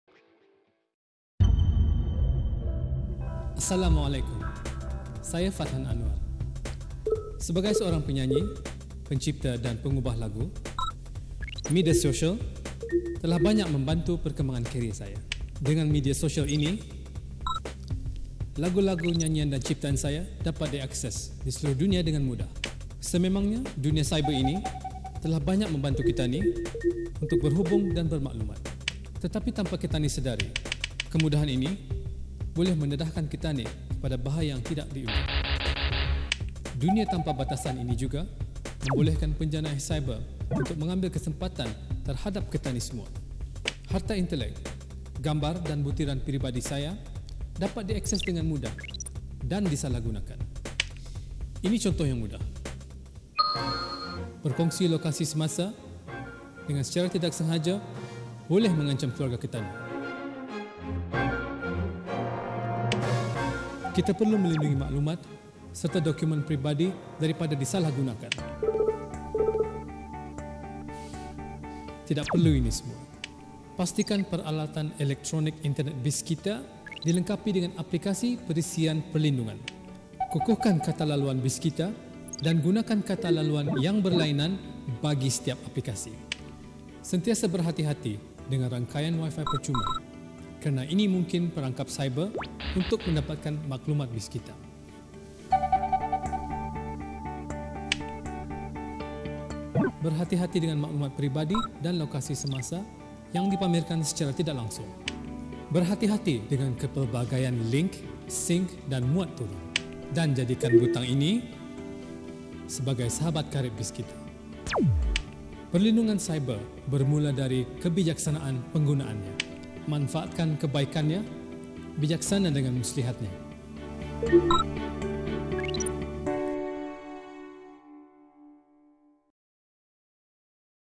Online Security with Fathan Anuar
A public service announcement by local Brunei artist Fathan Anuar on online security, shown on RTB3 HD.